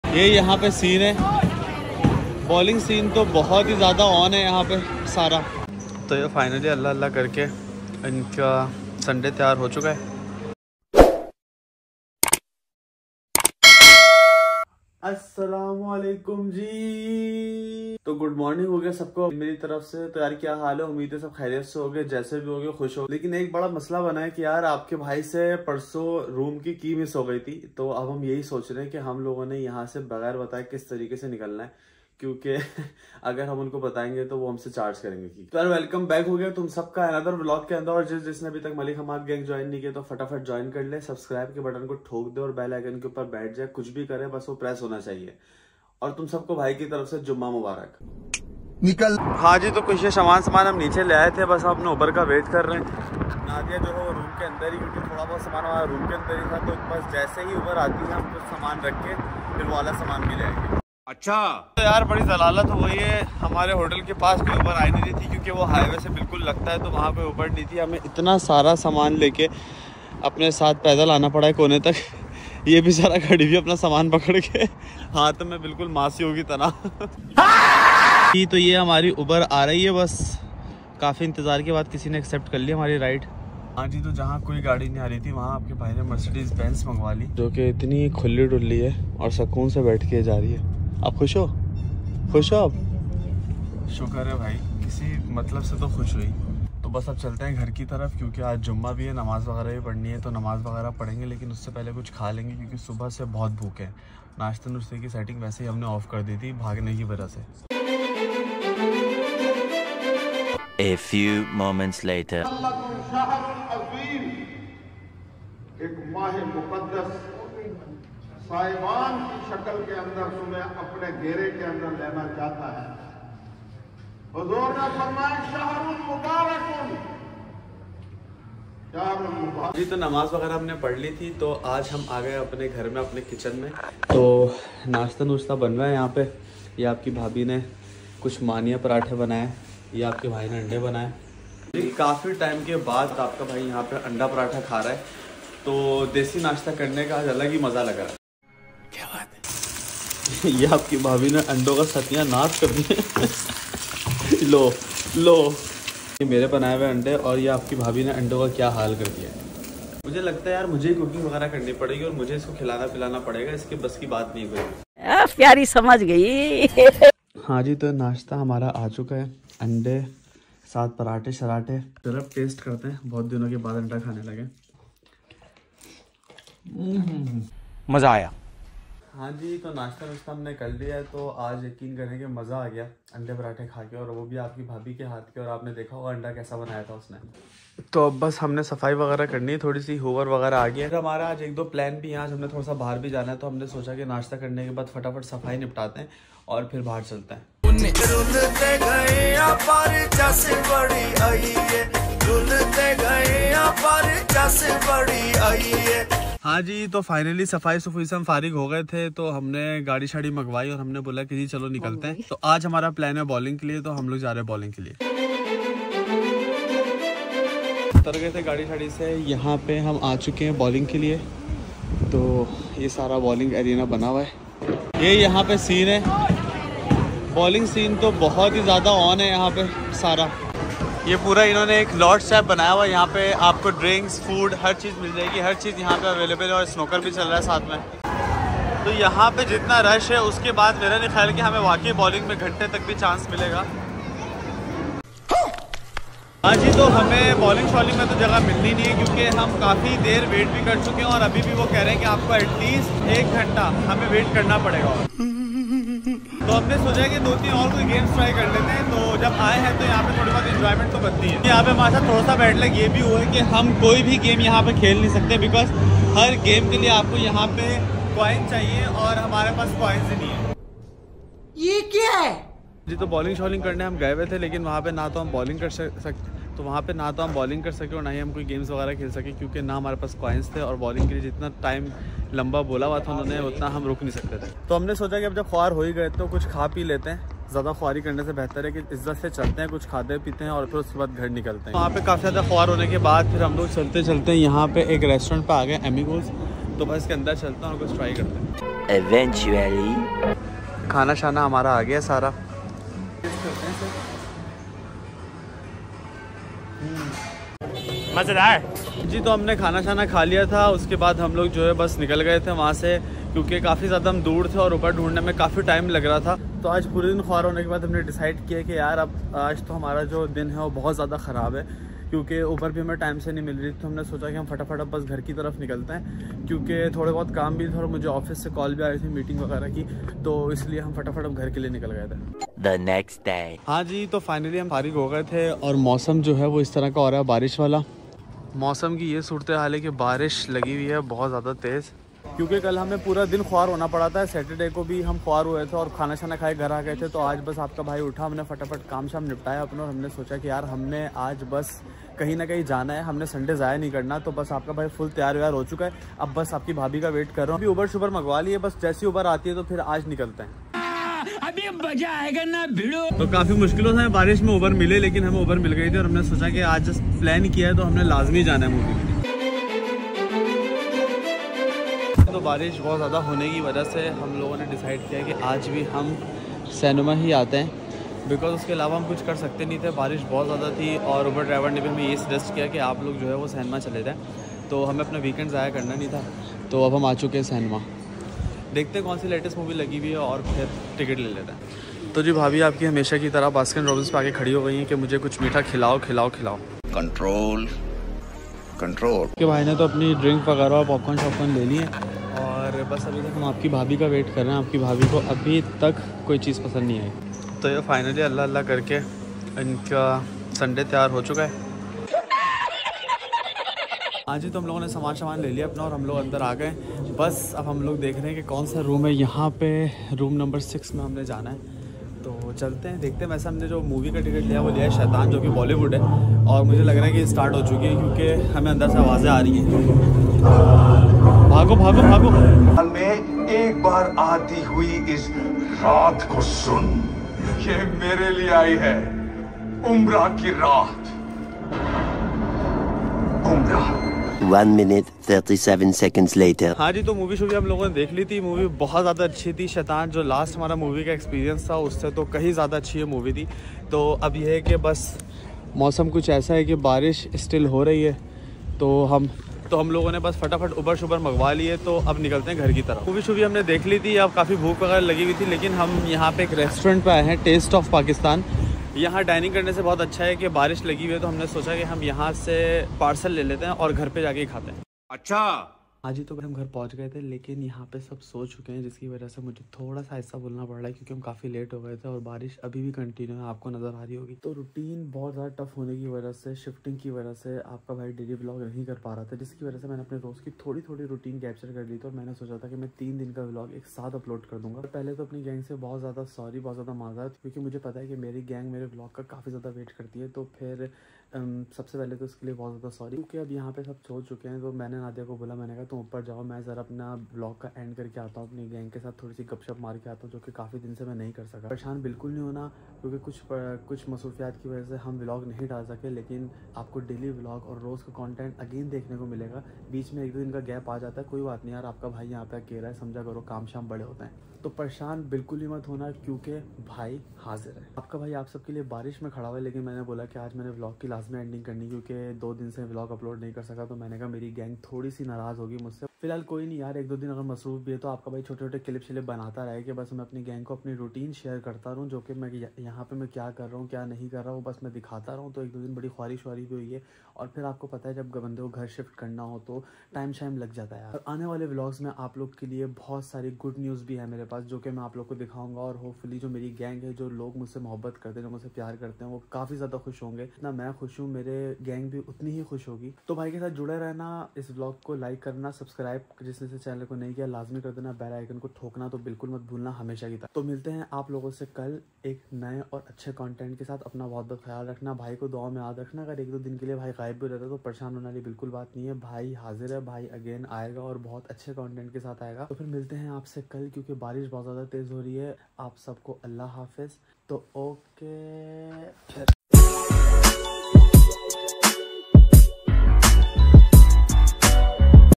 3 Din Ki Update Ek Sath🤐| Busy Days In London🥲| 20th Vlog
Masroofiyat Kafi Barhny Ki Waja Sy Vlog Late Hugya🥲 Pr Tension Nahe Leni Kiyun K "Bhaai Hazir Haaai" Thanks For The Support❤️ Keep Supporting ❤️